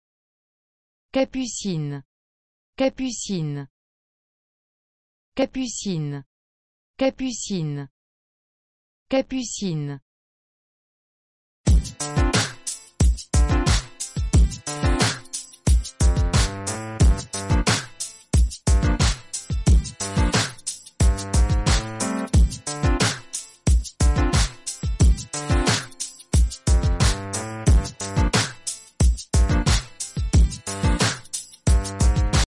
How do you say 'nasturtium' in French?
How to say 'nasturtium' in French? Listen to hear the pronunciation.